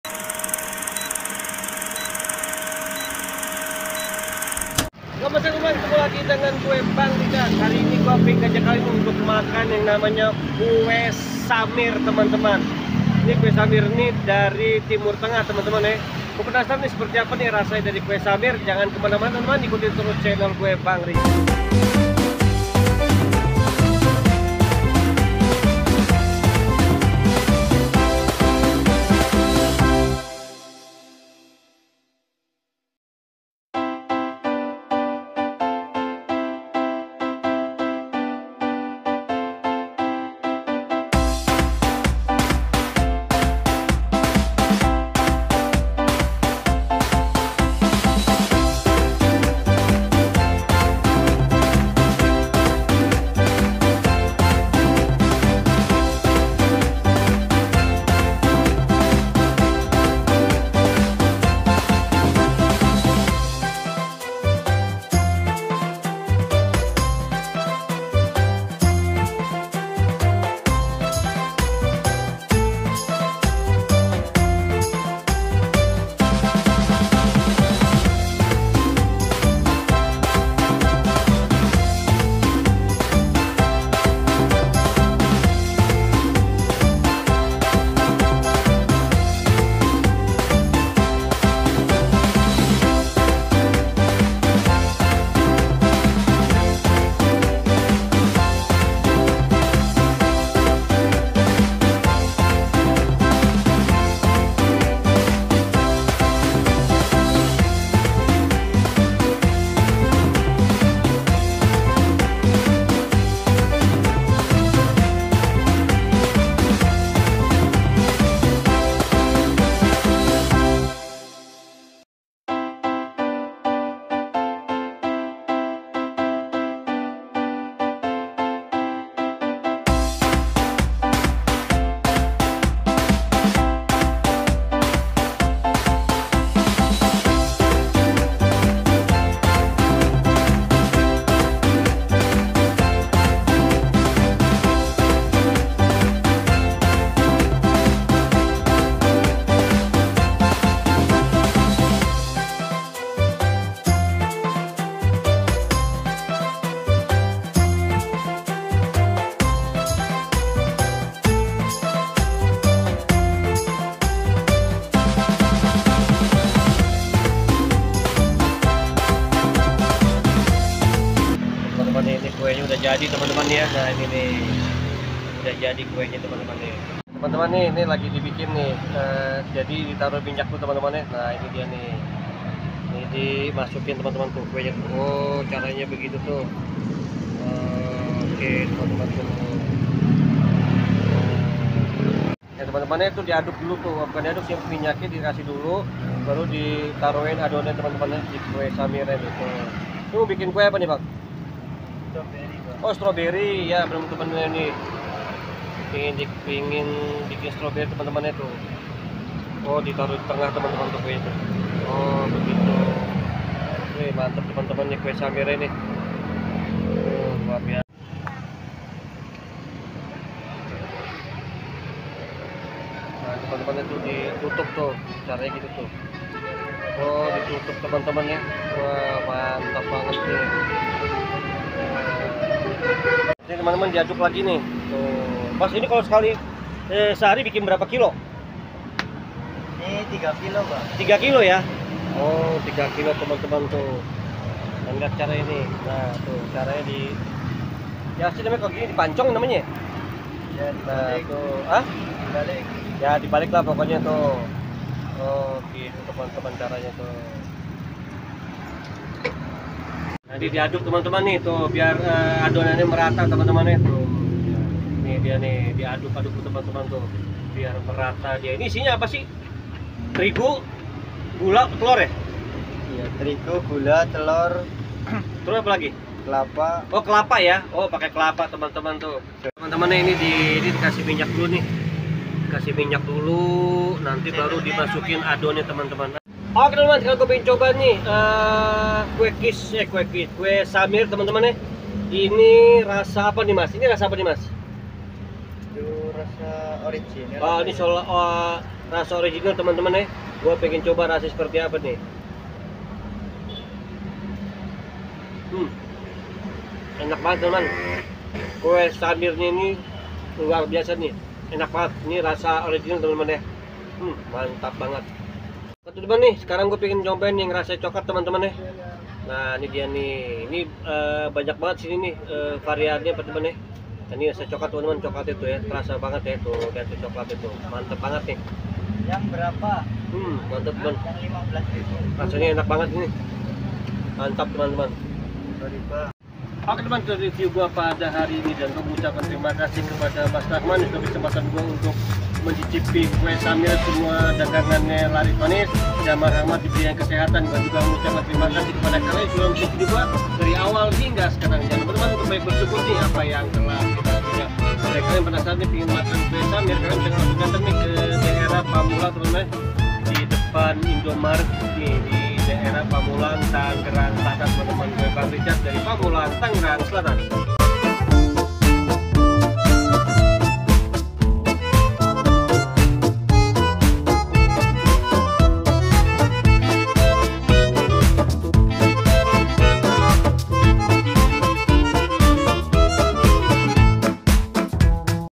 ENAK BANGET LOH!!! KUE SAMIR INI!!!!!!
hai ketemu lagi dengan gw bang Richard yuk kita cicipi kue samir yang berasal dari timur tengah wow.
 kue samir ini asalnya dari timur tengah dibuat dari tepung, telor, kelapa dll

terimakasih yang sudah menonton video vlog bang Richard
semoga kalian sehat selalu y